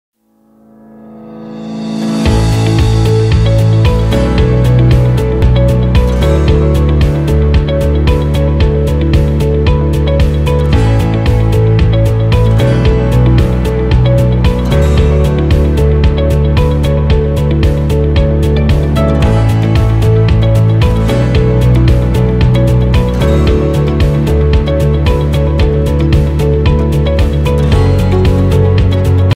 Double Wall Crash Lock Boxes | Davpack
Speed up packing and dispatch processes with these crash lock boxes. No tape or staples involved - simply pop up and you're good to go.